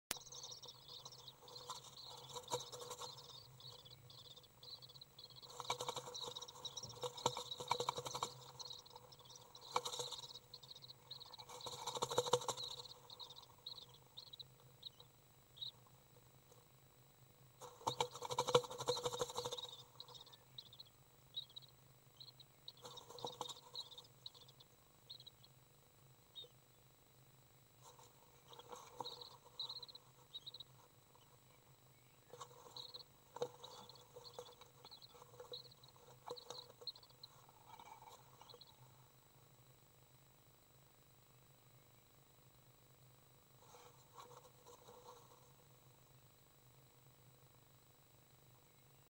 Common Swift flight-practise June 27th 2012
One of the young Common Swifts in my nest box no.1 practises flight by flapping and doing push-ups. More